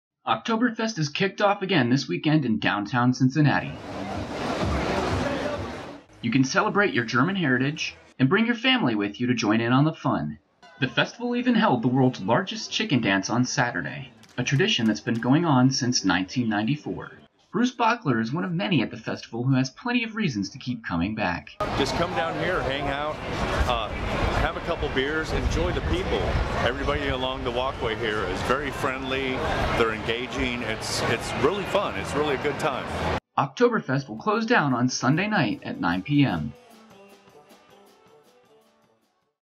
Oktoberfest Zinzinnati 2019
Video coverage of Oktoberfest Zinzinnati in 2019, a festival celebrating German heritage. The festival is taking place on the weekend of September 20-22, and closes on the 22nd at 9pm.

Footage captured and edited by Tyler Pistor on Saturday, September 21, 2019.

More information about my work, including my sports broadcasting demo reel, can be found on my